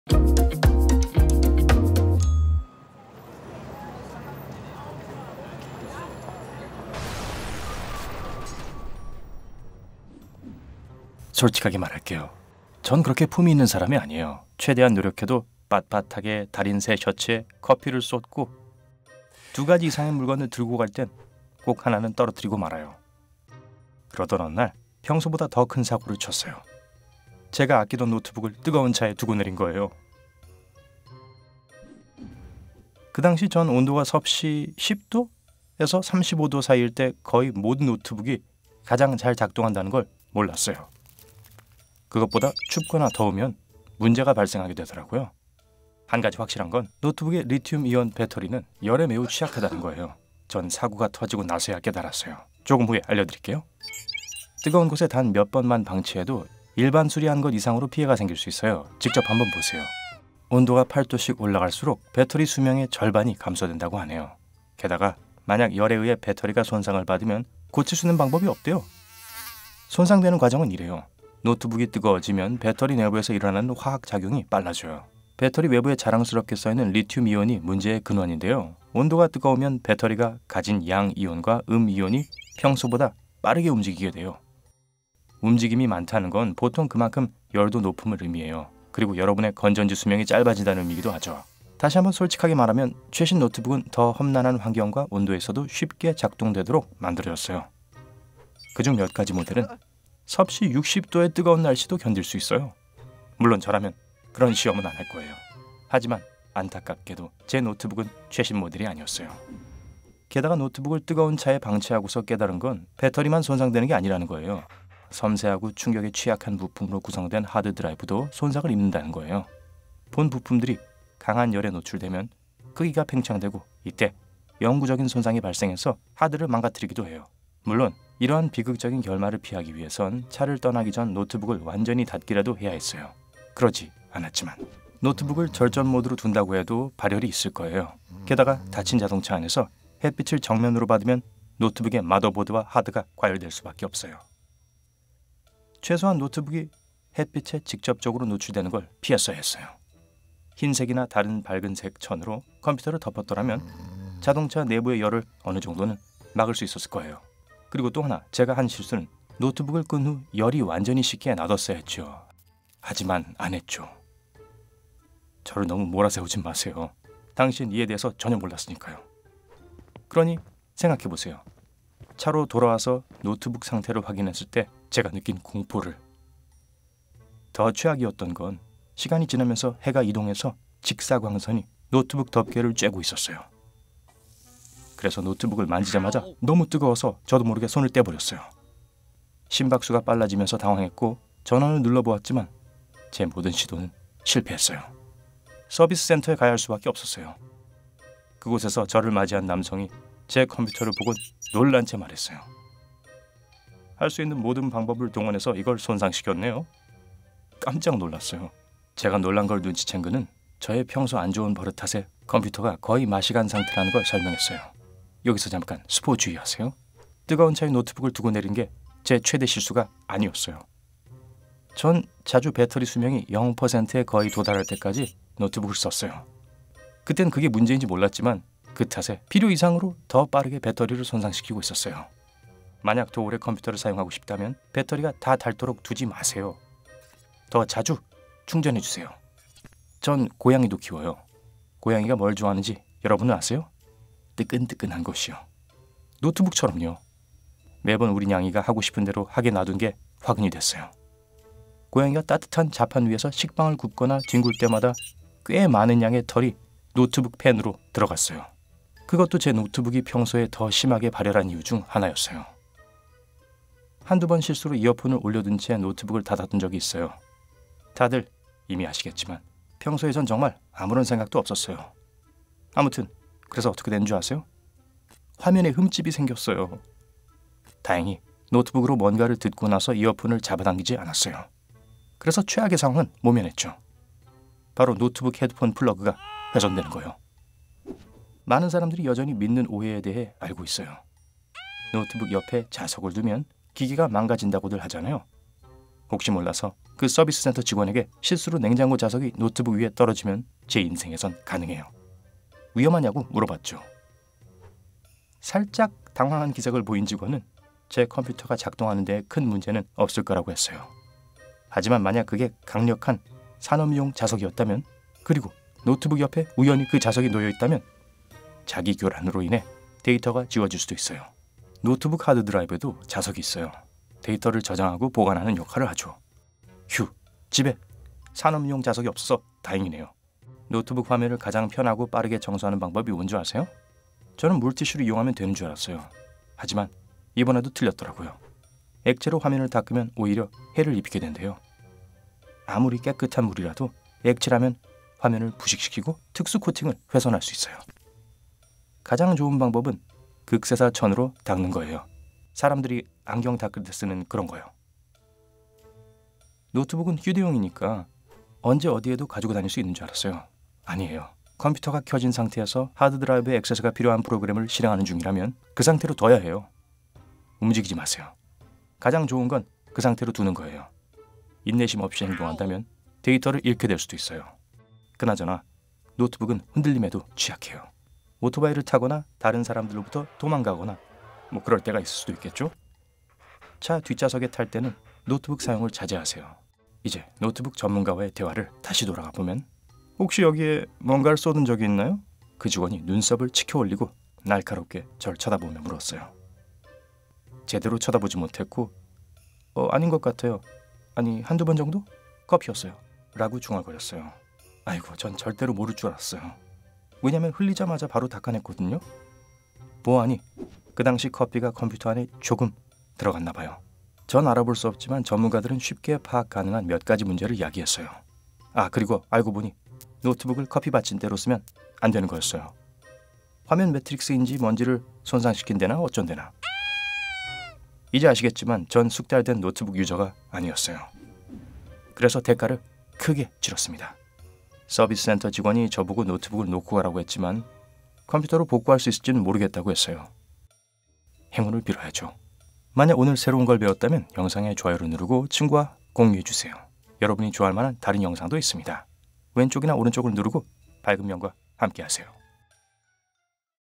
노트북을 차에 두지 마세요, 이렇게 될 지도 몰라요
혹시 온도가 섭씨 10도에서 35도 사이일 때 거의 모든 노트북이 가장 잘 작동한다는 걸 알고 있었나요? 그것보다 춥거나 더우면, 문제가 발생하게 되더라고요. 한 가지 확실한 건, 노트북의 리튬 이온 배터리는 열에 매우 취약하다는 거예요. 뜨거운 곳에 단 몇 번만 방치해도, 일반 수리하는 것 이상으로 피해가 생길 수도 있어요!

그나저나, 뜨거운 차에 노트북을 두면 배터리만 손상되는 게 아니에요. 섬세하고 충격에 취약한 부품으로 구성된 하드 드라이브도 손상을 입는 다는 거예요. 본 부품들이 강한 열에 노출되면, 크기가 팽창하게 되고, 이때 영구적인 손상이 발생해서 하드를 망가뜨리기도 해요. 그럼 어떻게 이런 비극적인 결말을 피할 수 있을까요?

타임스탬프:

사고는 이런 식으로 일어나요 👈 1:08

손상될 수 있는 다른 것들 1:52

영구적 손상 피하는 법 2:07

노트북을 망가뜨리는 것들 💥 3:42

고양이는 안 돼요! 4:01

노트북 화면 닦는 법 5:30

우리 체널을 구독하세요~~~